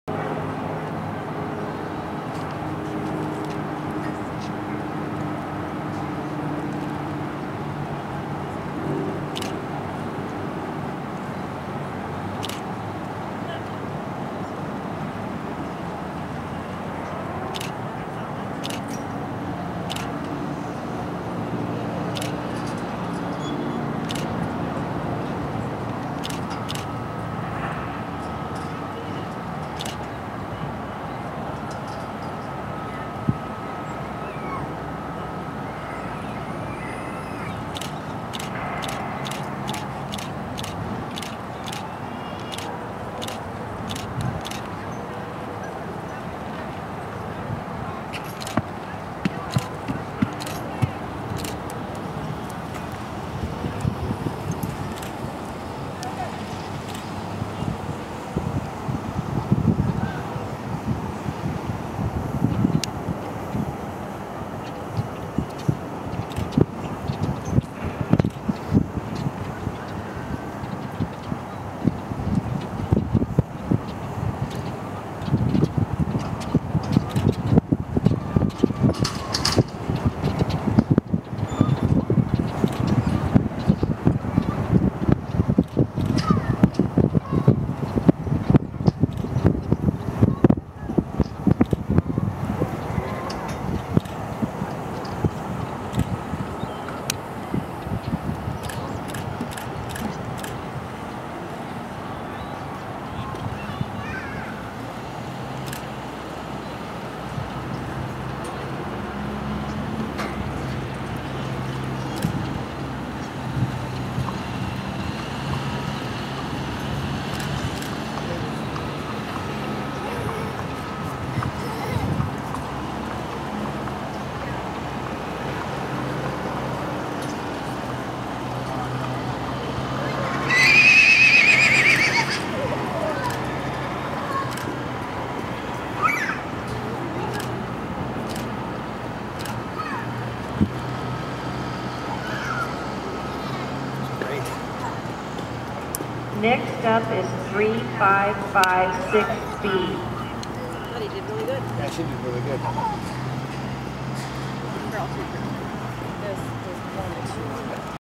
2015 Interstate Fair: Trillium trail in-hand
2015 Interstate Fair with Trillium Knight
Trail in-hand: 3 yrs & over

This is Trillium's first year with the big horses (he is 3). He placed 4th out of at least 13 in the class.